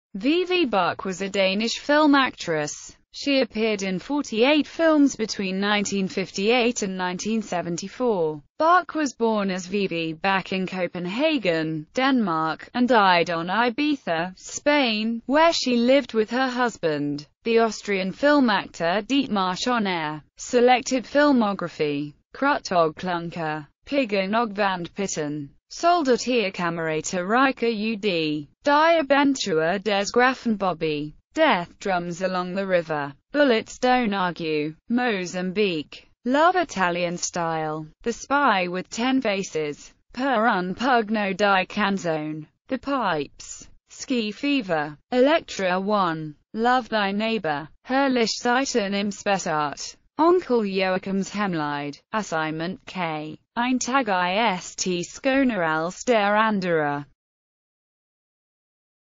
Vivi Bach
Vivi Bach (3 September 1939 – 22 April 2013) was a Danish film actress.She appeared in 48 films between 1958 and 1974.